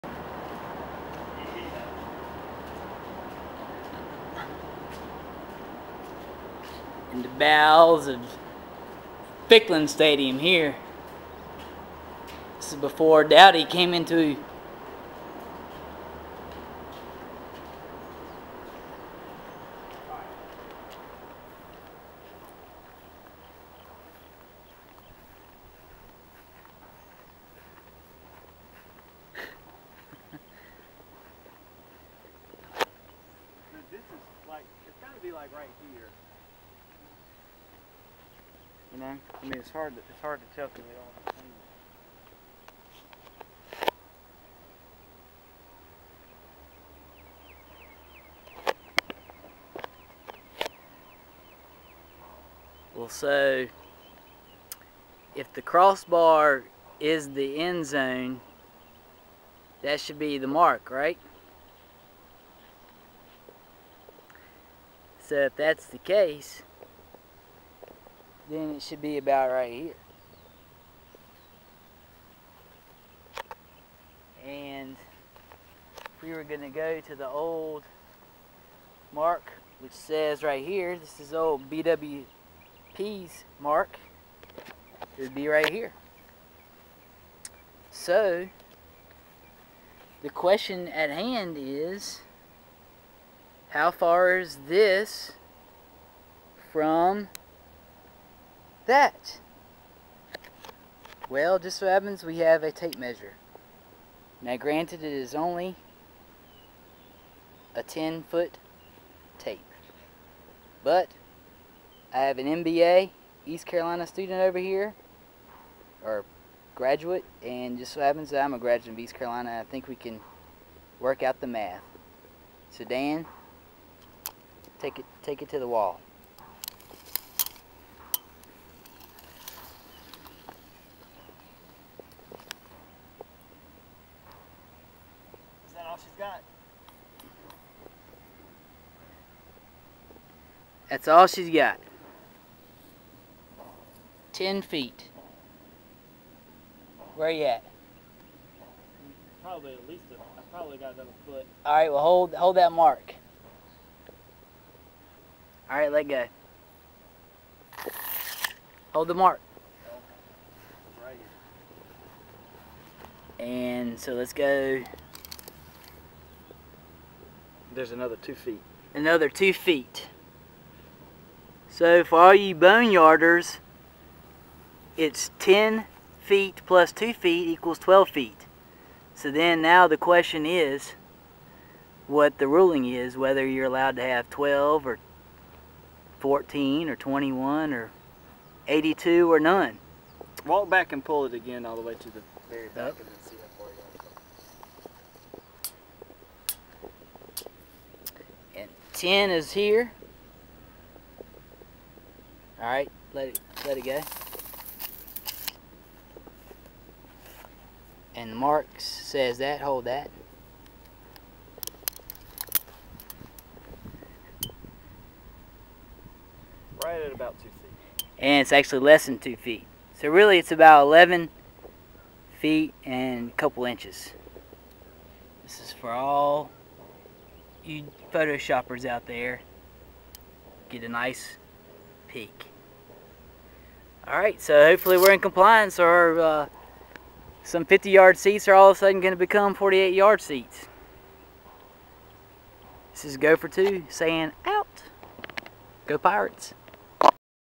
Dowdy-Ficklen Endzone Measurement
Unofficial measurement of Northeast Corner of Dowdy-Ficklen Stadium-Greenville, NC May 2010